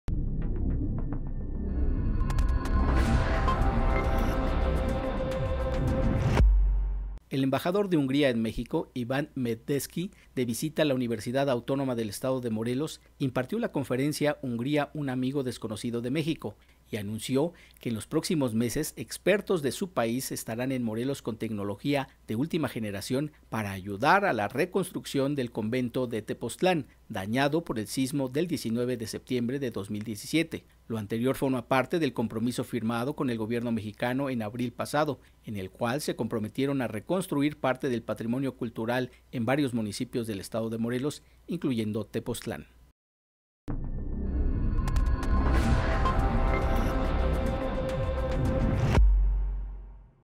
Hungría ayudará en la reconstrucción del patrimonio cultural de Morelos
Ciudad Universitaria a 13 de junio de 2019.- El embajador de Hungría en México Iván Medveczky, de visita a la Universidad Autónoma del Estado de Morelos (UAEM), impartió la conferencia “Hungría un amigo desconocido de México”, en el auditorio del Centro de Investigación en Dinámica Celular (CIDC), y anunció que en los próximos meses expertos de su país estarán en Morelos con tecnología de  última generación,  para ayudar a la reconstrucción del Convento de Tepoztlán, dañado por el sismo del 19 de septiembre de 2017.
Lo anterior forma parte del compromiso firmado con el gobierno mexicano en abril pasado, en el cual se comprometieron a reconstruir parte del patrimonio cultural en varios municipios del estado de Morelos, incluyendo Tepoztlán.

Dirección de Comunicación Universitaria

Voz e información: Oswald Alonso
Imágenes de video: Elia García
Fotografías: Lilia Villegas
Producción de audio: Griselda Navarro
Edición de video: Nelly Ramírez